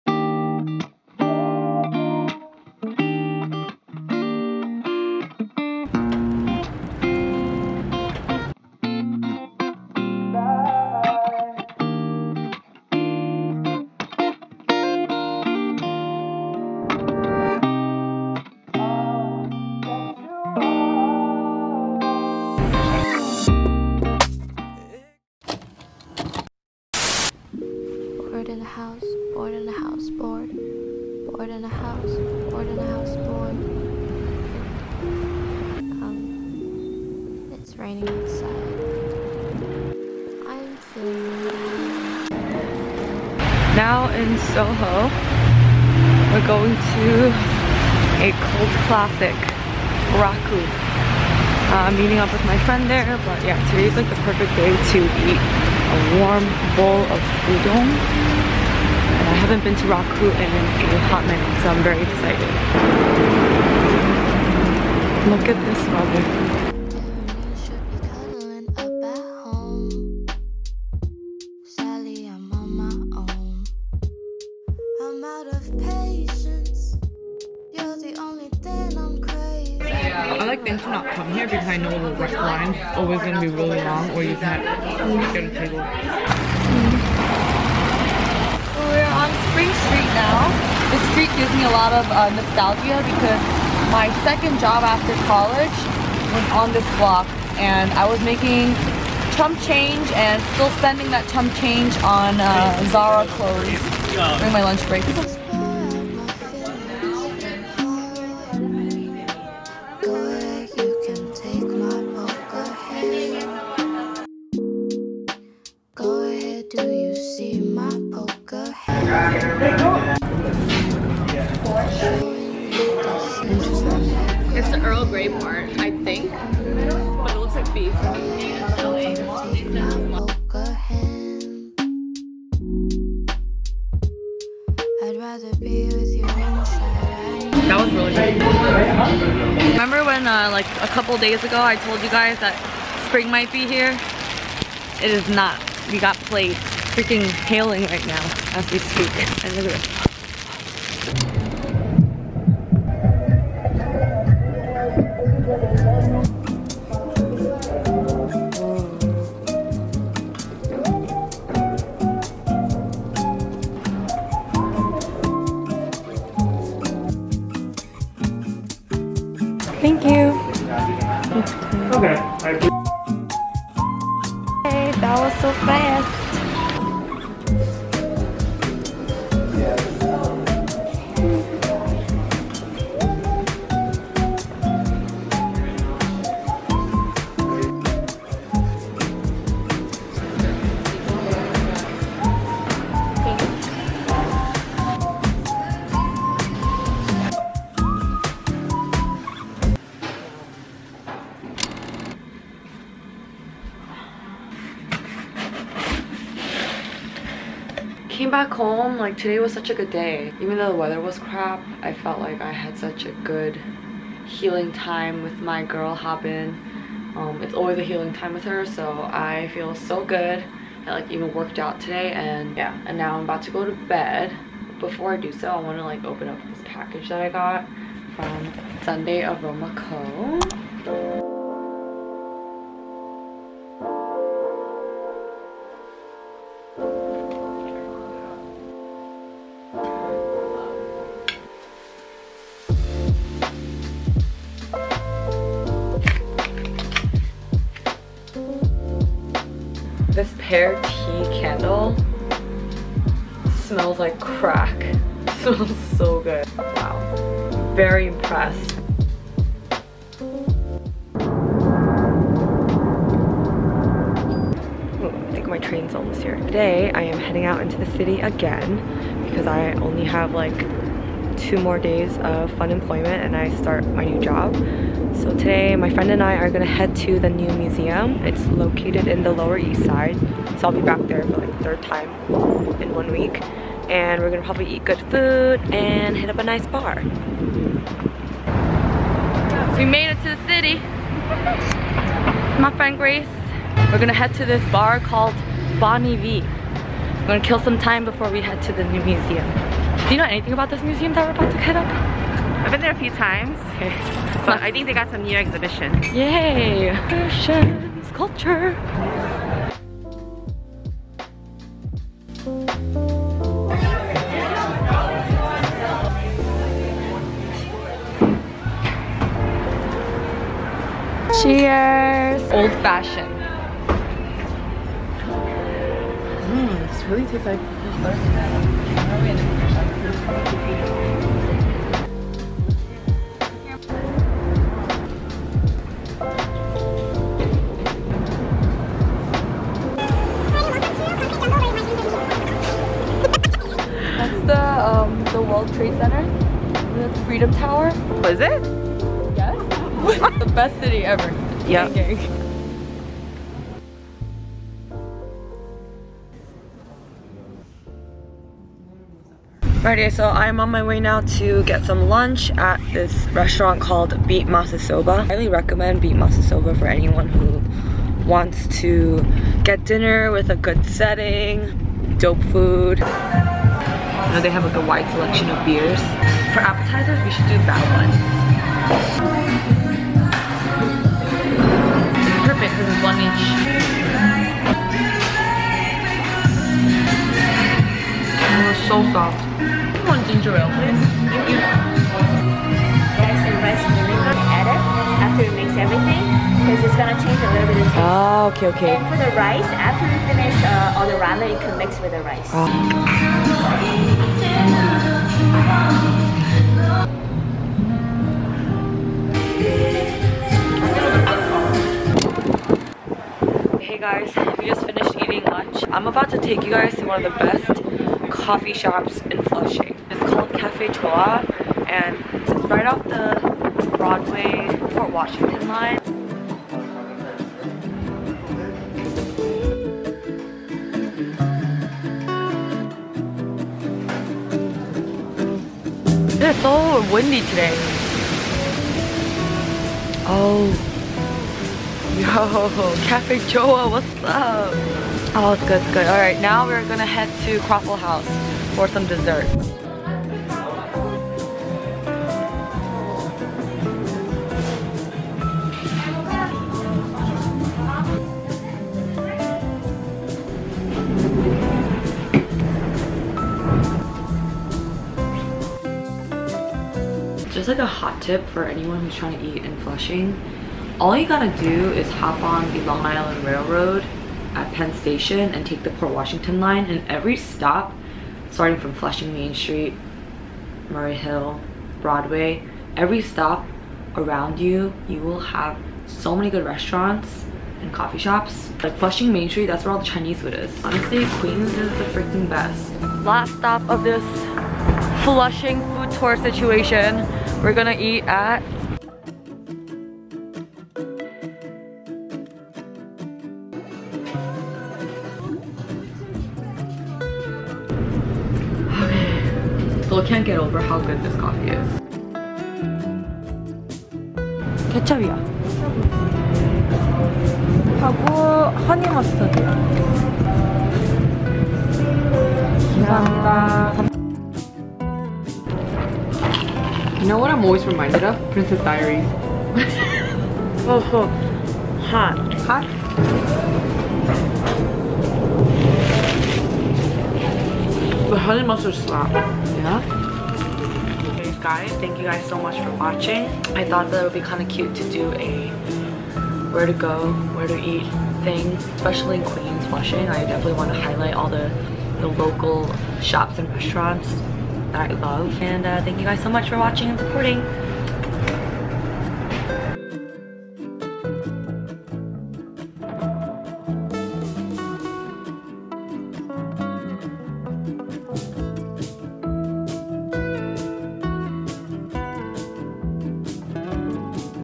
nyc vlog | where to go + places to eat pt. 3 ✨
Haven’t done one of these in awhile :p  Had some time to explore the city  highlight a few of my favorite local restaurants & spots in NYC!

+ mentioned
raku (manhattan)
dominique ansel bakery (manhattan)
the hideout chai bar (manhattan)
luster photo & digital (manhattan)
harney & sons (manhattan)
bonnie vee (manhattan)
new museum (manhattan)
sunday aroma co (ny based brand)
beat mazesoba (queens)
cafe joie (queens)
croffle house (queens)
ugly donuts & corn dogs (queens 150 st)

CODE: LOPARK10

CODE: LOISPARK